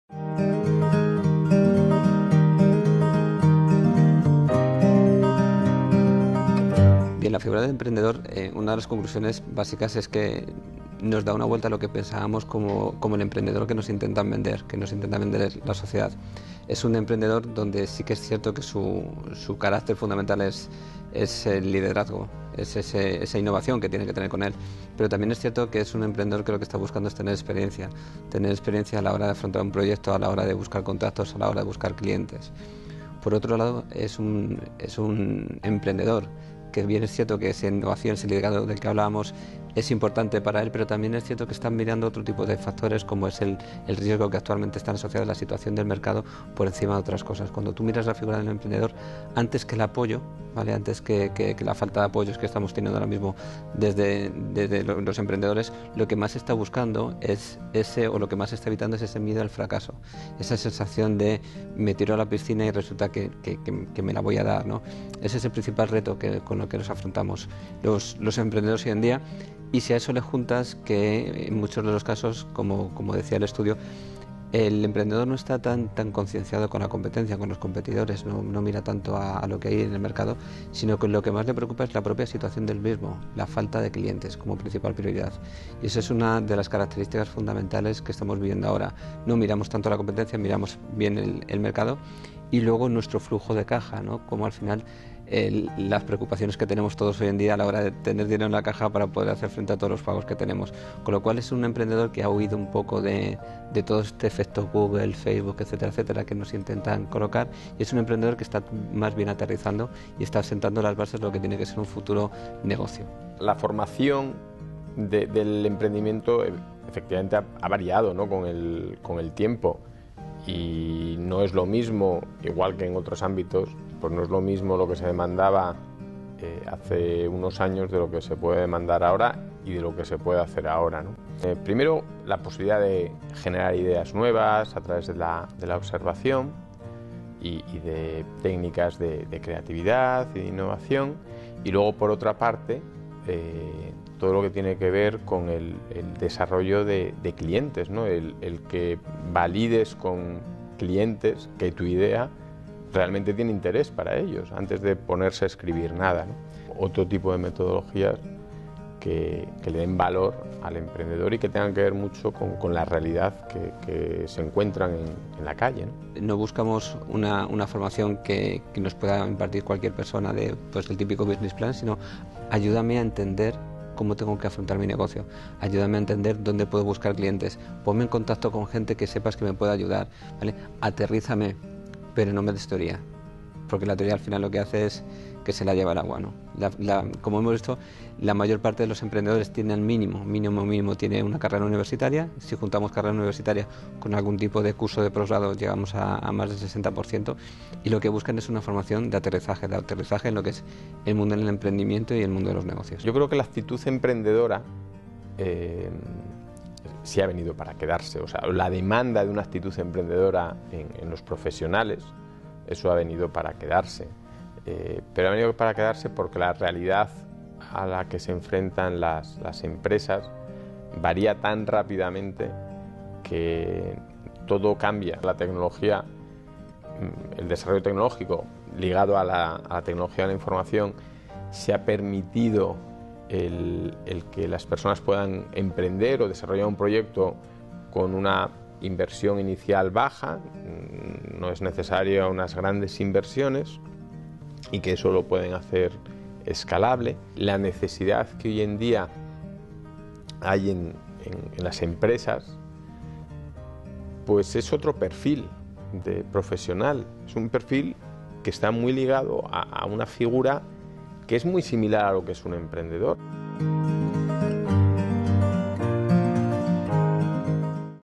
Entrevistas sobre el Estudio de Emprendedor en Internet 2014
Entrevistados:
Valentín Hernández, Fundador My Observer
Ramón Gurriarán, director de Competitividad  y Emprendimiento - EOI
Estudio del Emprendedor en Internet
Noviembre de 2014 - Mes del Emprendedor en EOI